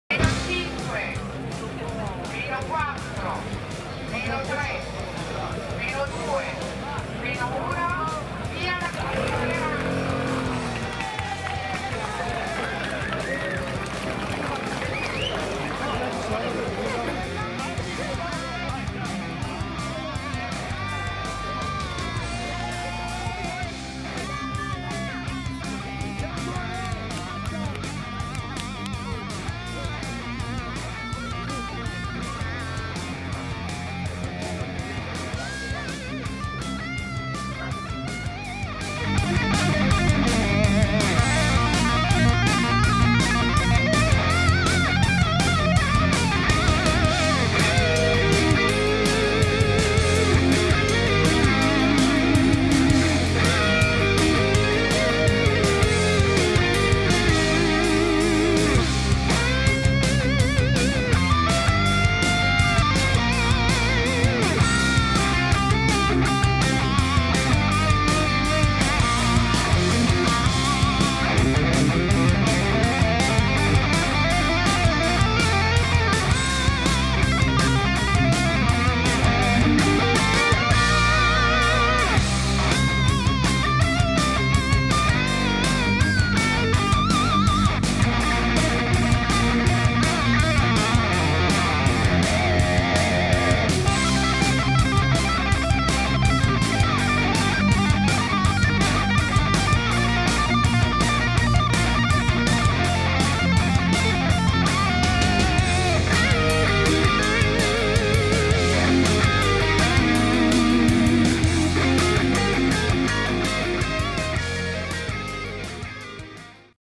SaleRun 2016 - Partenza gara podistica
Partenza della V edizione della gara podistica non competitiva SaleRun del 23 ottobre 2016 a Salerano Canavese (Torino) che si pone l’obiettivo di raccogliere fondi per l’Associazione Casainsieme Hospice e per il nascente centro Alzheimer. 9 Km attraverso i comuni di Salerano, Banchette, Fiorano e Samone all'insegna dello sport e della solidarietà.